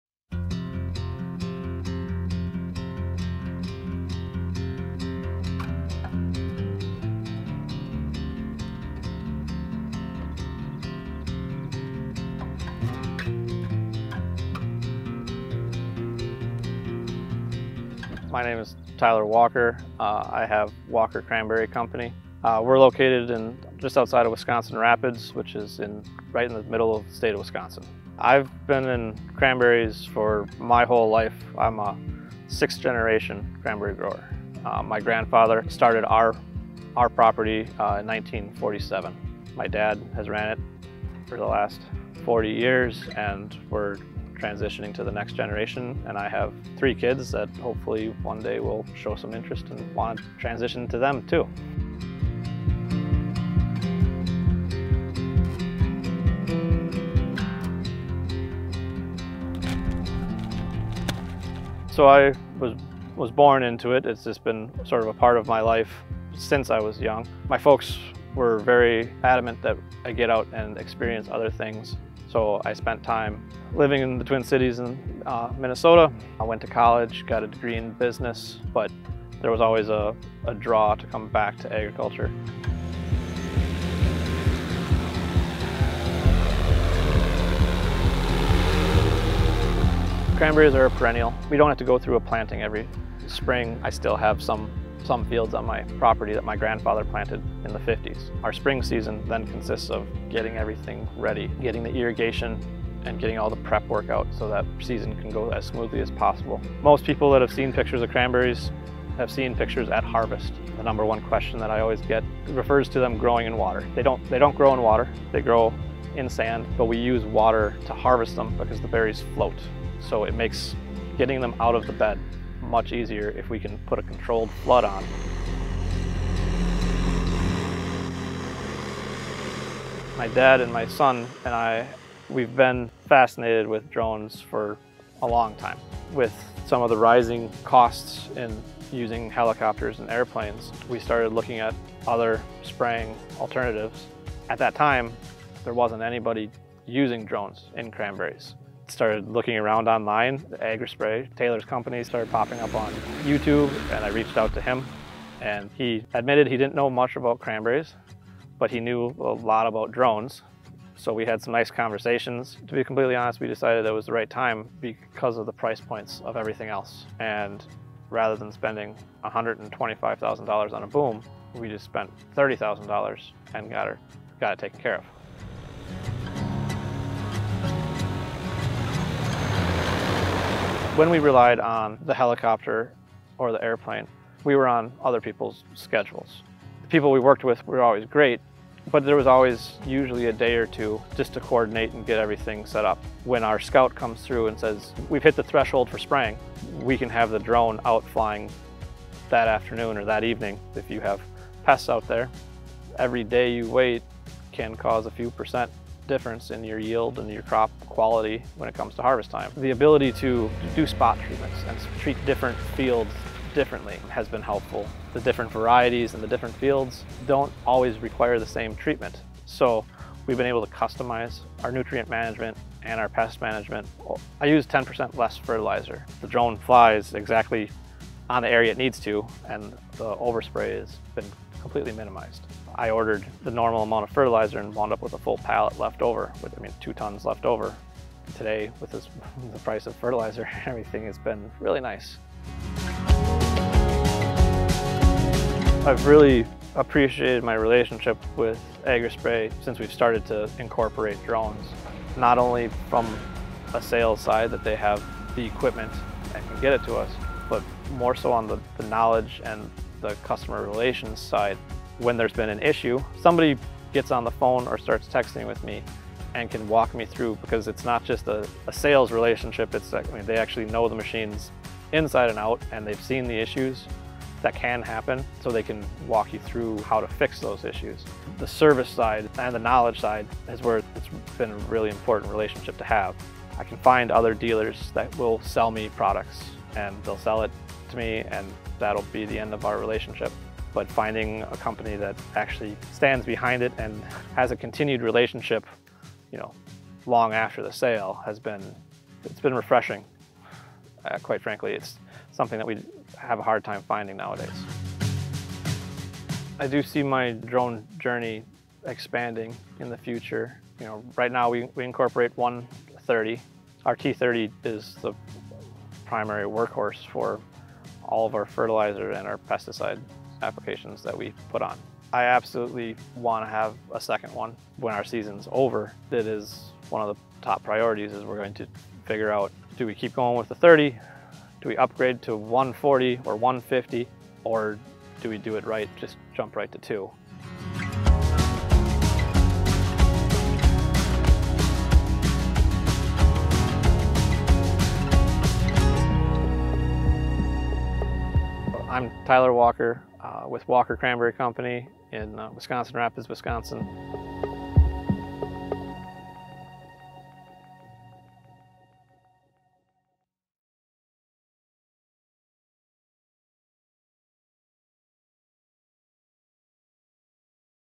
Walker Cranberry Company | Customer Spotlight - Agri Spray Drones
Join Tyler Walker from Walker Cranberry Company in Wisconsin as he shares how spray drones transformed the cranberry farm of six generations.

Tyler shares impact the DJI T30 had in revolutionizing operations, offering cost-effectiveness and breaking free from traditional constraints.

Learn how the drone's flexibility optimizes responses to changing conditions, allowing precise treatments tailored to specific field needs and how he was able to significantly reduce cost in fertilizer applications by cutting down on waste.